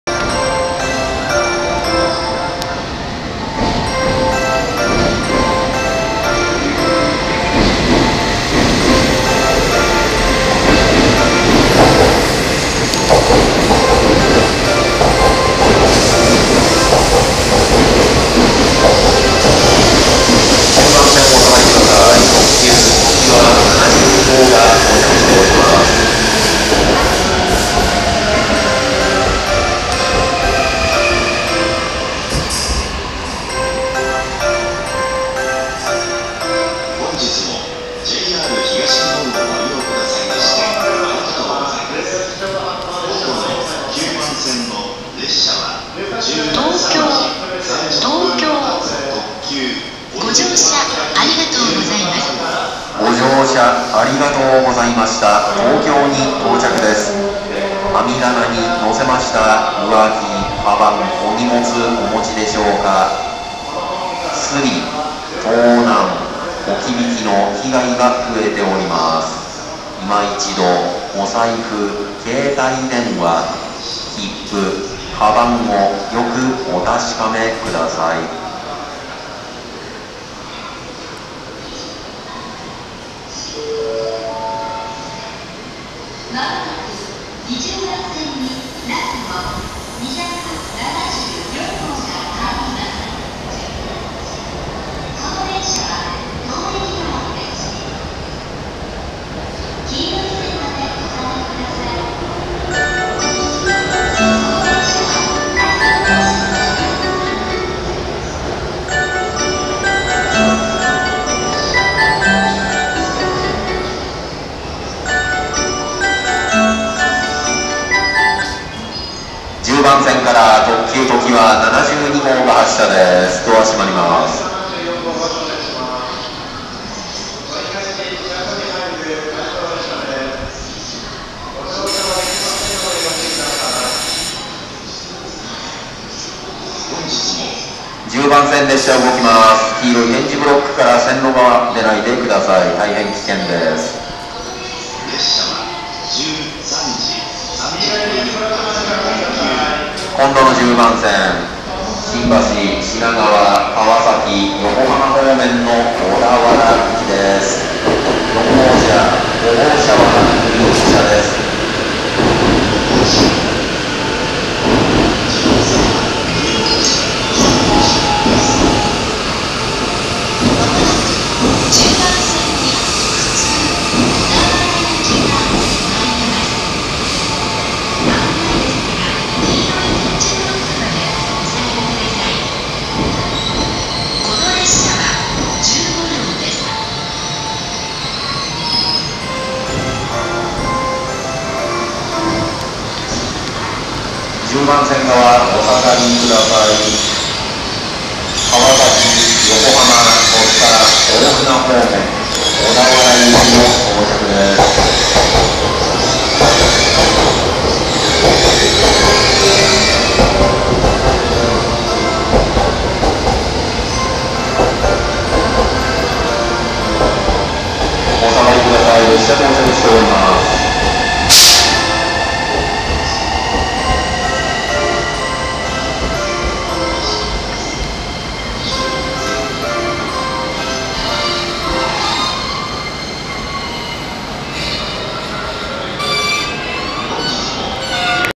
JR東京駅から東北新幹線なすの259号郡山行きE5系U35と常磐線特急ときわ72号品川行きE651系が発車！東北新幹線なすの274号東京行きE5系U25、E6系Z7、普通小田原行きE231系が到着！
JR東京駅10番線ホーム上野駅側から撮影！
東京駅20番線ホームから、下り10両編成東北新幹線なすの259号郡山行きE5系U35E514-35（10号車)、E523-35(1号車)の新幹線が上野駅に向けて発車！

東京駅10番線ホームに、上野駅を発車して走行中の、上り10両編成常磐線特急ときわ72号品川行きE651系の電車が到着！2駅先の次の停車駅の終点の品川駅に向けて発車！

東京駅20番線ホームに、上野駅を発車して走行中の、上り17両編成東北新幹線なすの274号東京行きE5系U25E523-25、E514-25E6系Z7E611-7、E621-7の新幹線が到着！

東京駅10番線ホームに、上り15(10＋5)両編成上野東京ライン東海道線直通普通普通小田原行きE231系の電車が到着！

E5系
製造　川重
車両製造　平成24年(2012年)4月